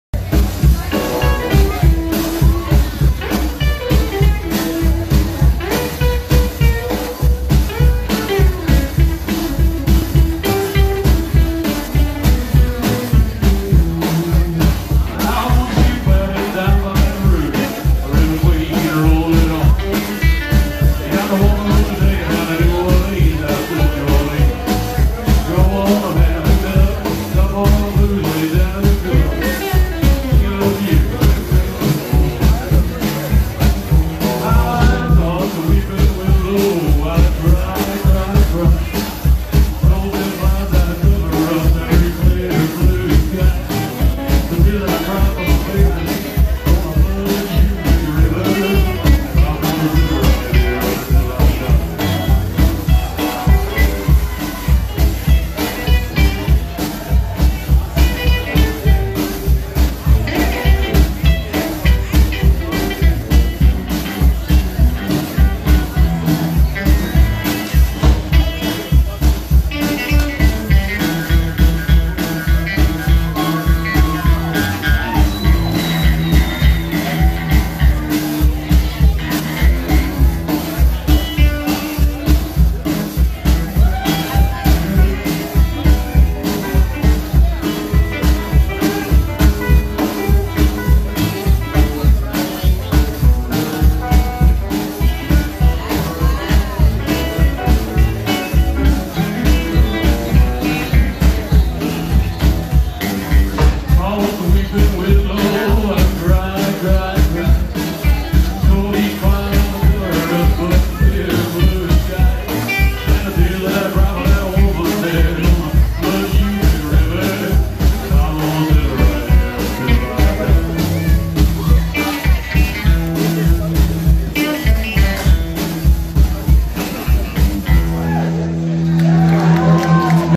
Eddie Clendening & The Blue Ribbon Boys and Friends
Eddie Clendening - Vocals, Guitar
Matt Codina - Guitar
Todd Wulfmeyer - Upright Bass
Joel Paterson - Guitar
Robert Greenig - Drums

Reverend Martini Presents
All Night Jumping Showcase
Viva Las Vegas Rockabilly Weekend 24

The Orleans Hotel and Casino